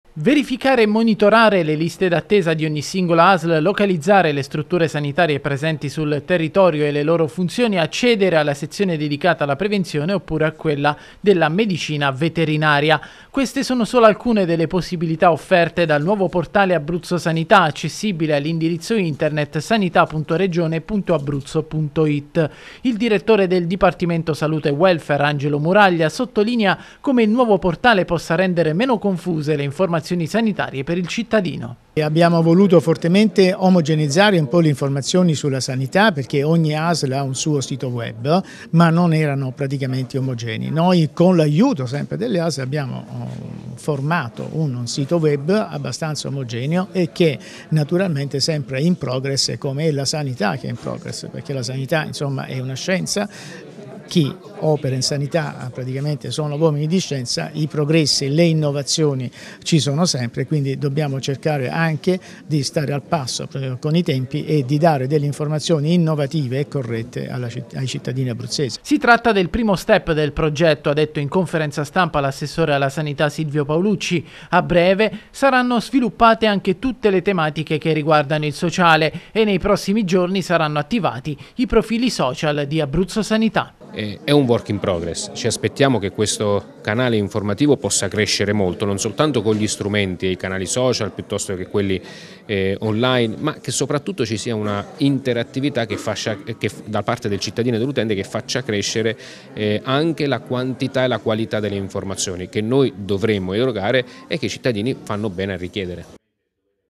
On line il portale Sanità Abruzzo
La sanità abruzzese ha un nuovo portale internet dal quale i cittadini avranno accesso alle informazioni sanitarie relative alla 4 Asl regionali